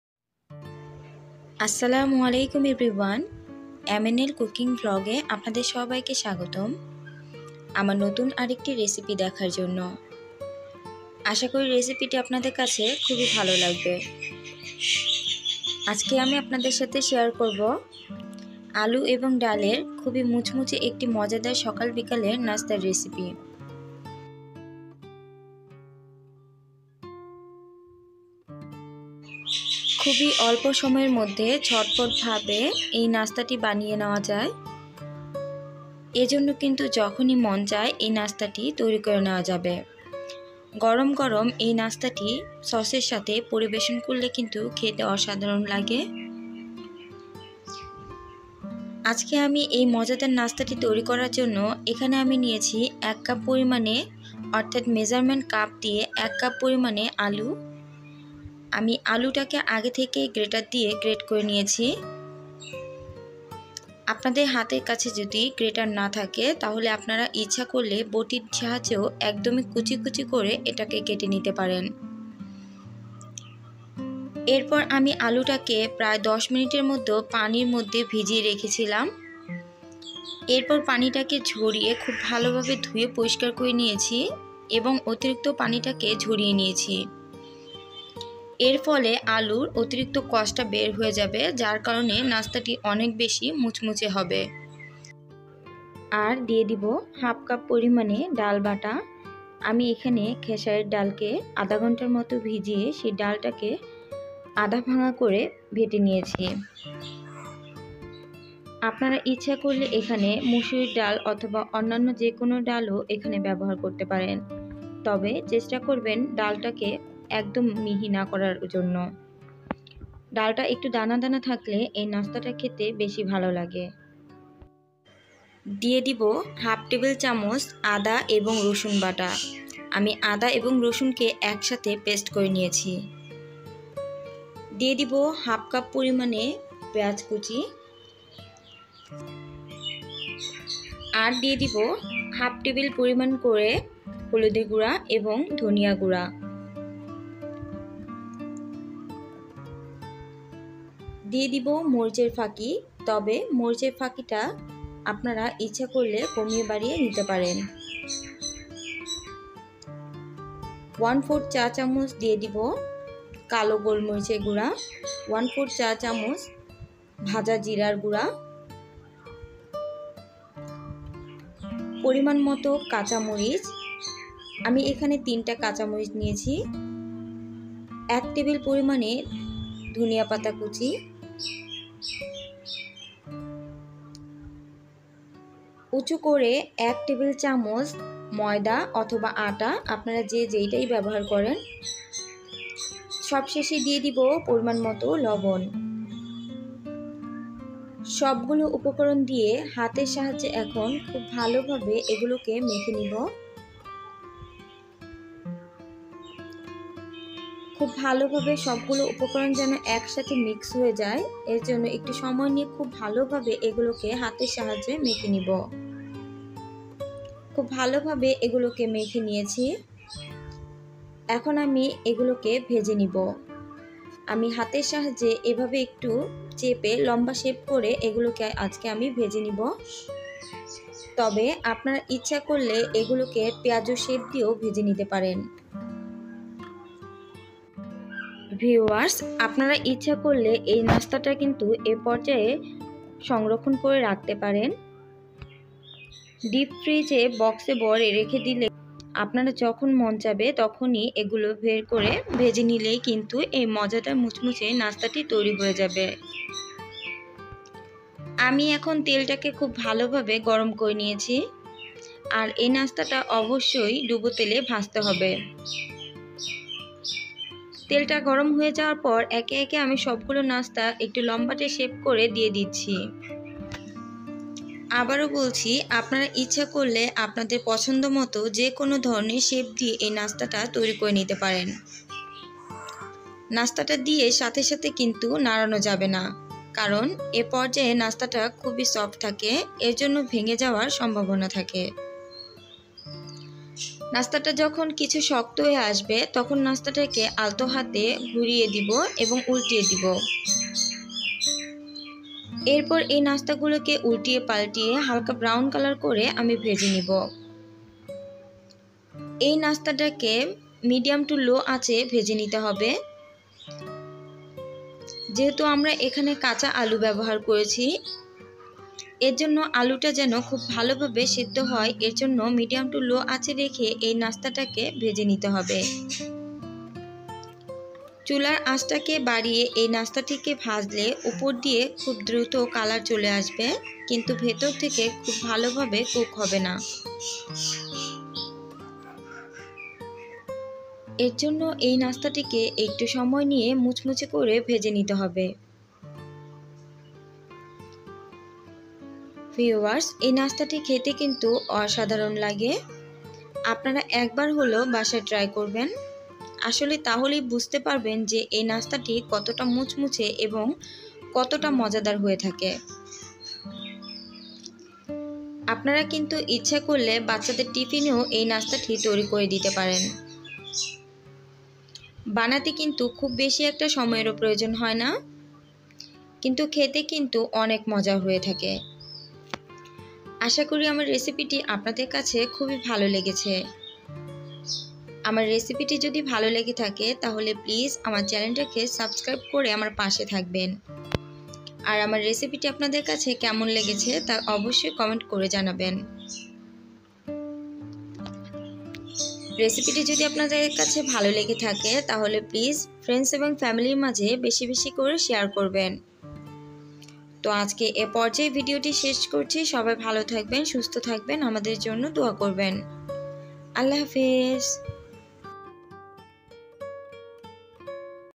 Nasta||snacks recipe||ডাল আর আলুর তৈরি মুচমুচে নাস্তাটি একবার খেলে বারবার খেতে ইচ্ছে করবে
Nast||snacks recipe.ডাল আর আলুর তৈরি মুচমুচে নাস্তাটি একবার খেলে বারবার খেতে ইচ্ছে করবে||nasta

উপকরণ
আলু
 ডাল